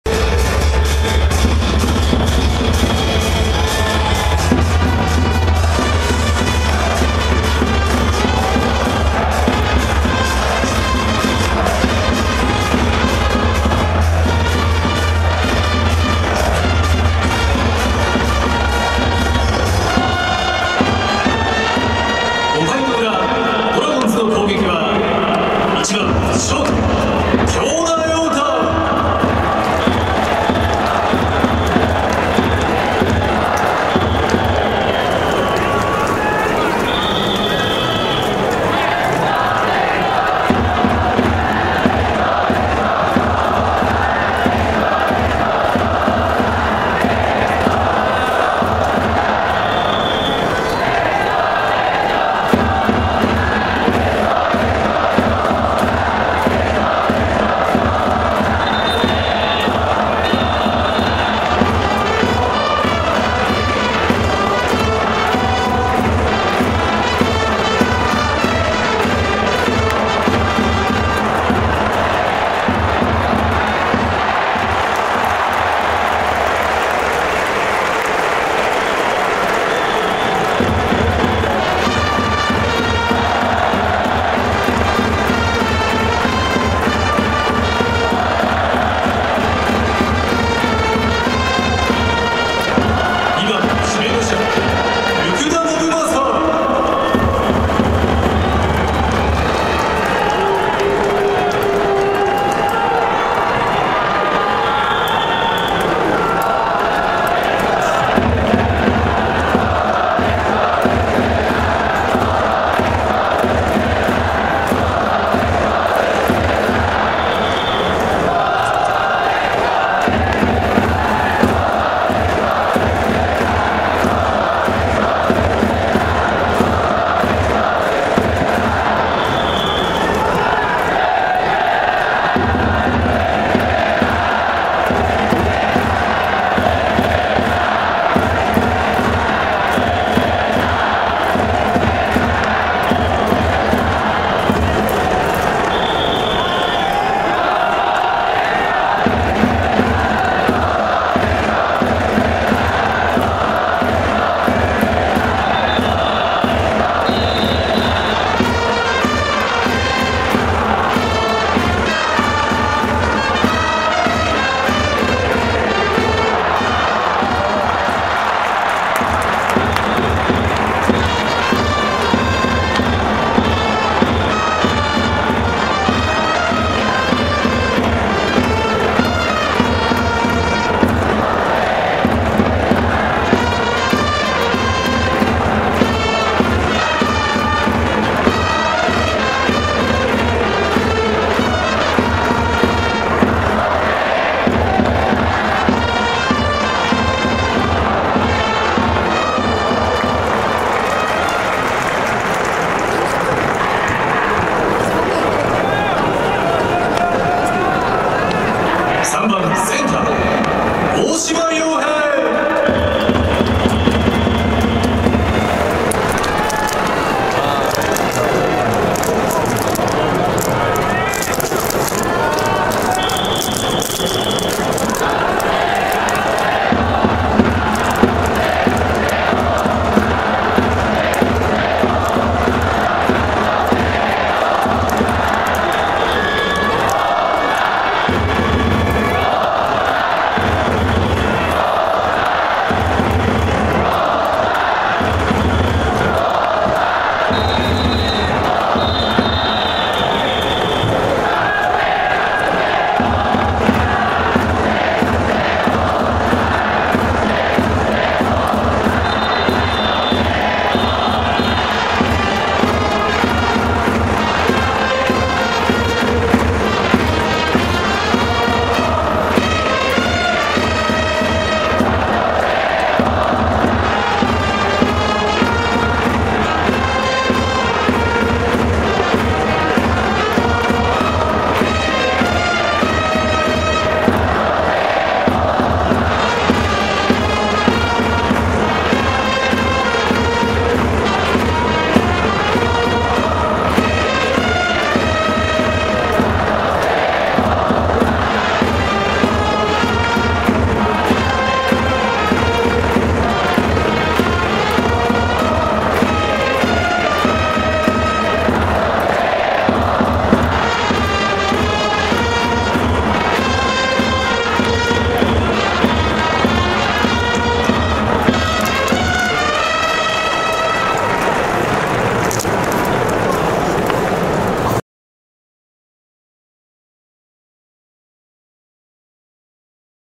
中日－千葉ロッテ ５回裏（2019年03月03日日曜）
２０１９年３月３日日曜
１４：００～１７：０５（３時間５分）

プロ野球オープン戦　荒木雅博選手引退試合
中日ドラゴンズ－千葉ロッテマリーンズ

備考
音声のみの公開となります。